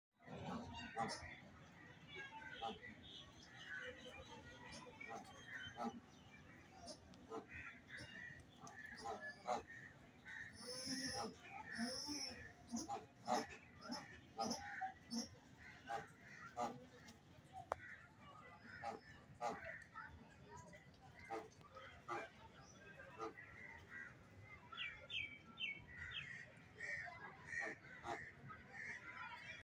Top 10 Beautiful |Canadian Goose
Today you can see amazing canadian goose farm in jhung pakistan